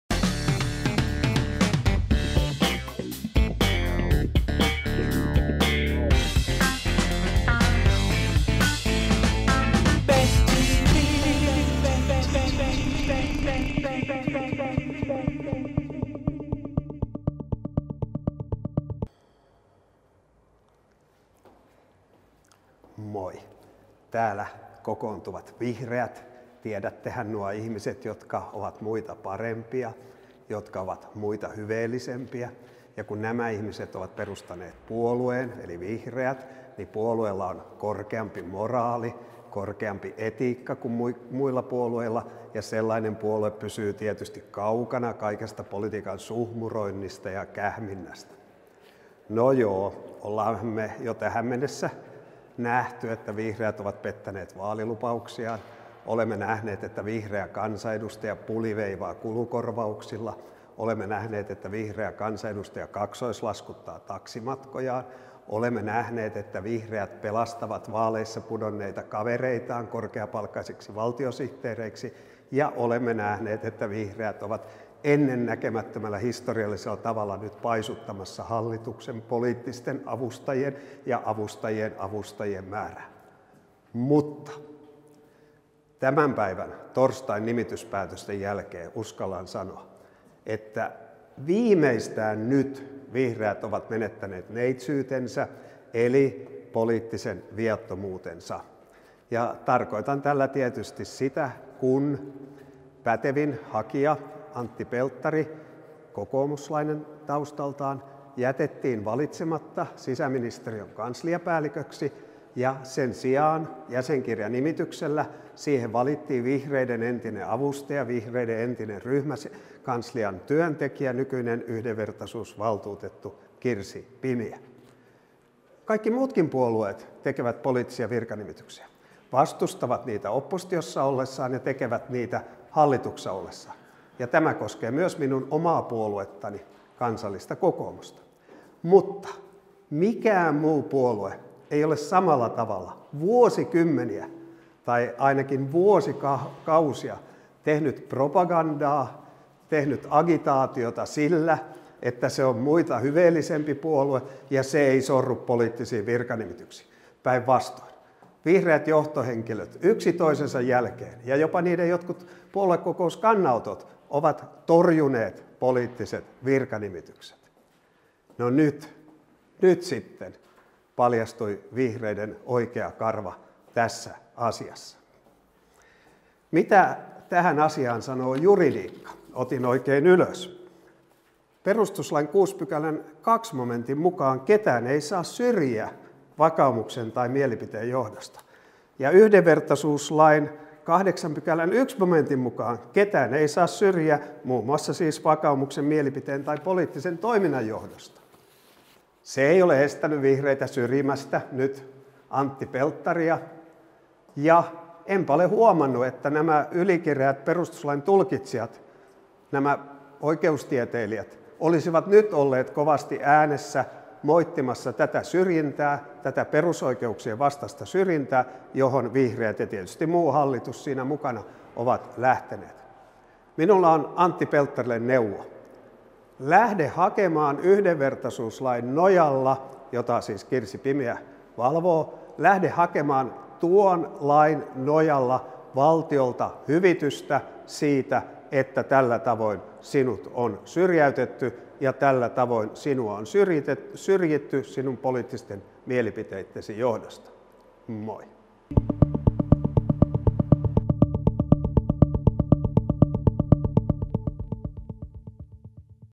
BenTV: Vihreiltä meni neitsyys
Nyt nähty poliittinen virkanimitys vei vihreiltä viattomuuden.